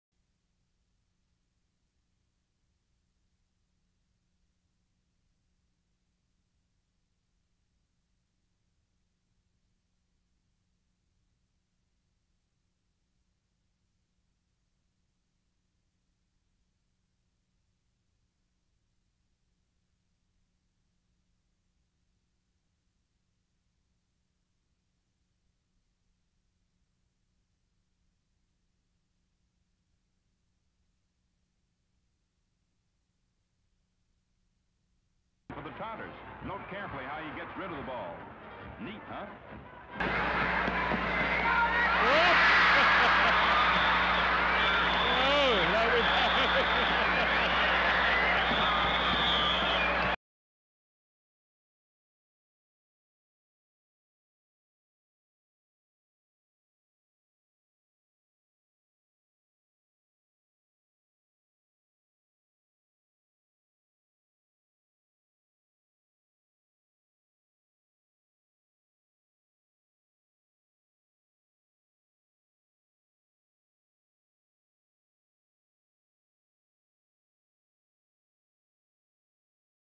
Harlem Globetrotters Vintage Footage Widescreen 1920x1080
Harlem Globetrotters Vintage Footage in conjunction with a Southern California Harlem Globetrotters Ticket Giveaway.